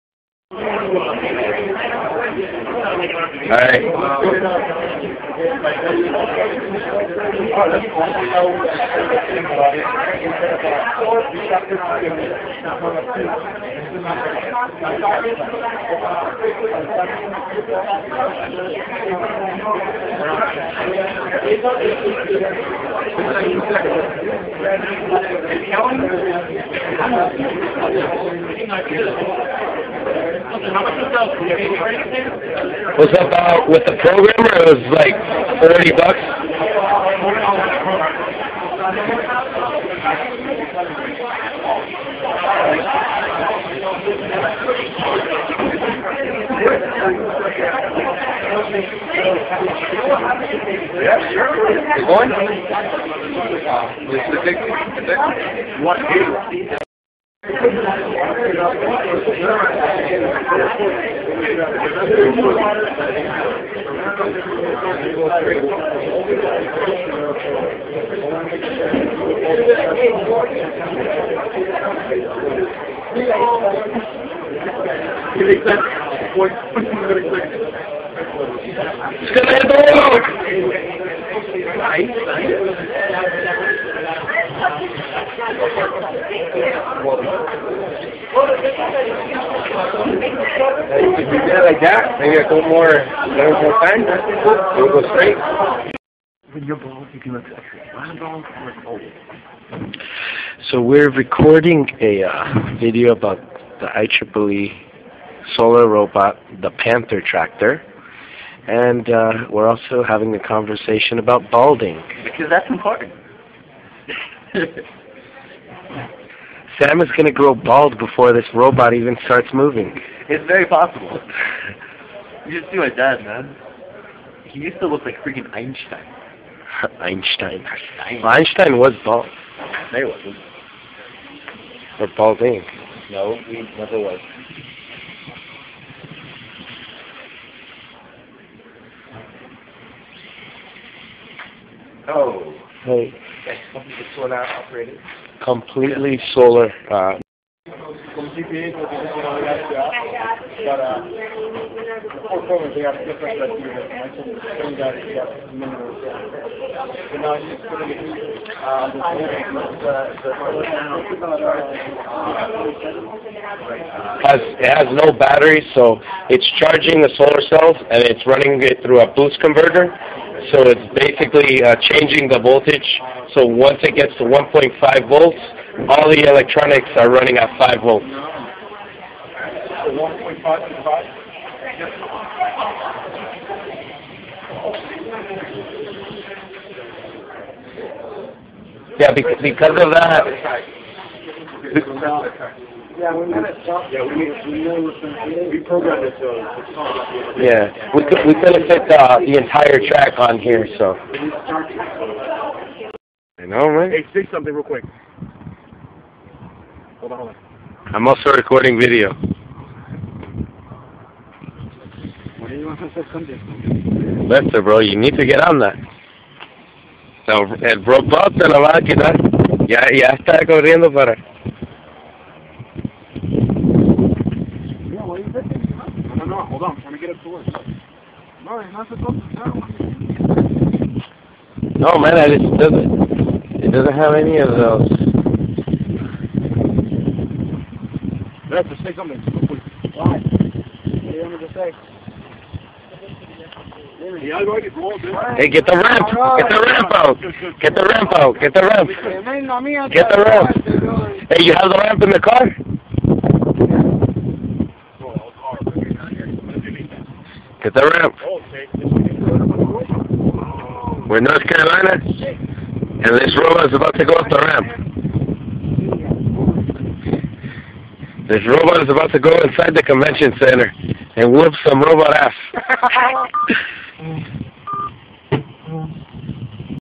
Solar Robot IEEE FIU
FIU's Solar Robot, the "PANTHER TRACTOR" which competed in IEEE Southeastcon 2010 in Charlotte, NC. This robot runs on 100% Solar Energy, NO BATTERIES AT ALL. One of the few robots that was capable of scaling the ramp on the course.